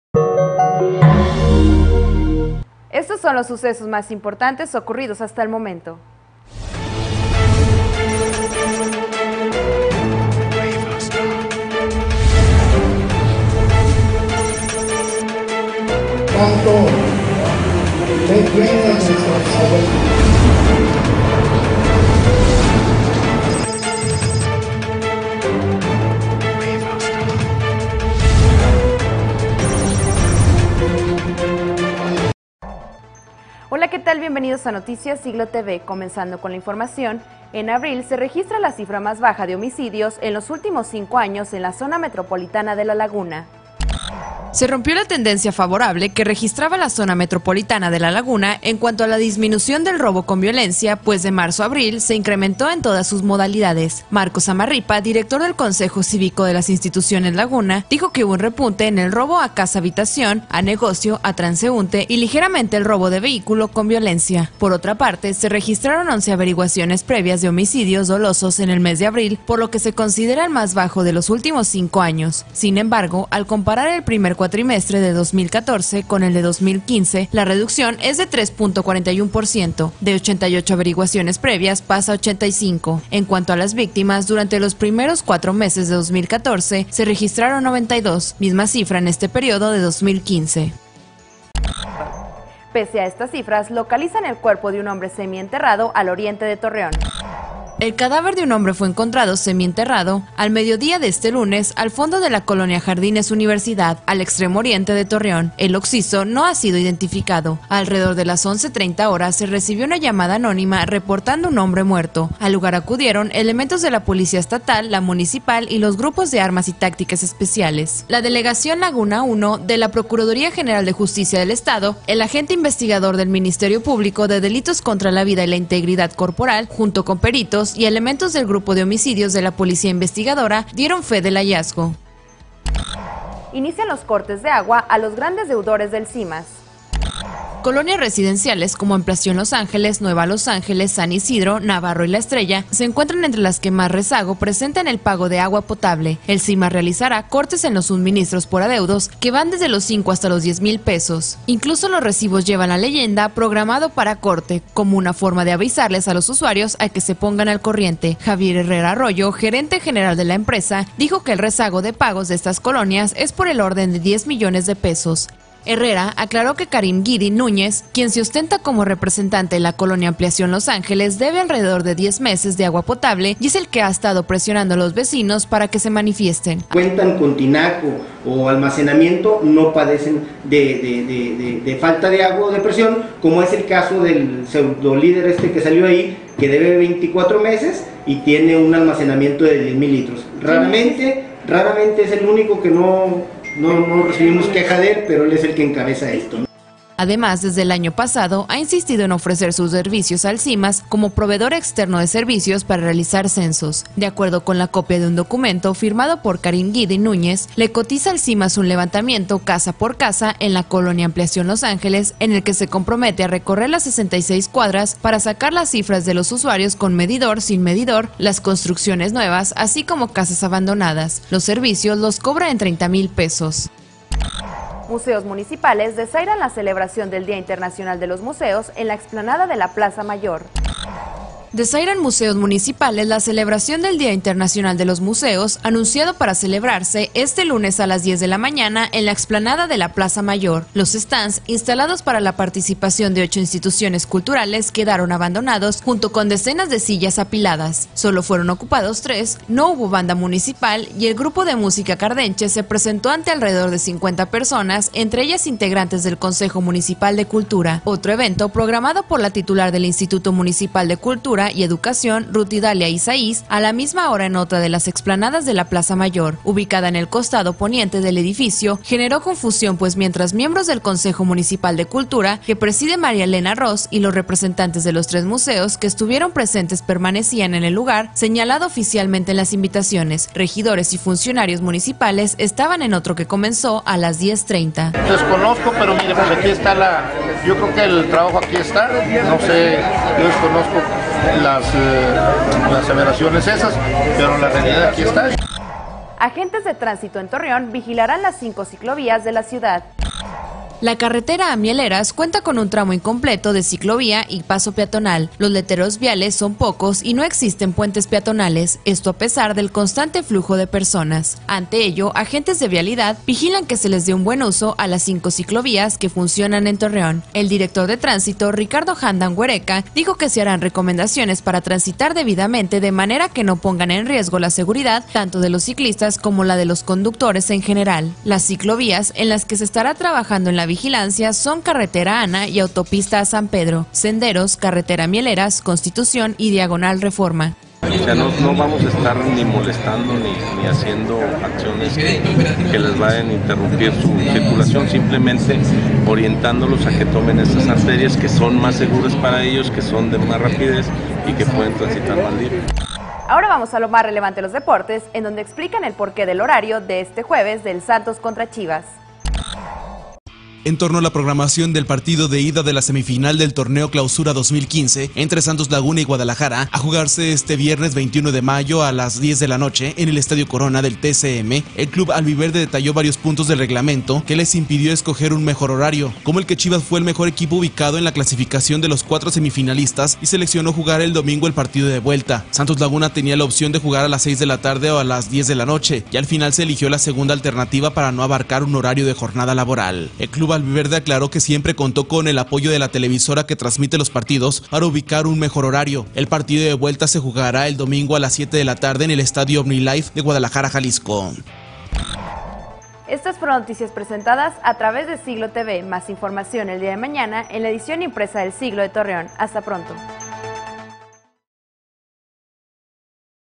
Registra Zona Metro cifra más baja de homicidios
Localizan cadáver de hombre semi enterrado al oriente de Torreón ::: Inician cortes de agua a grandes deudores de Simas ::: Museos municipales desairan celebración en la Plaza Mayor ::: Agentes de tránsito vigilarán las ciclovías ::: Justifican horarios del partido de semifinal Santos vs Chivas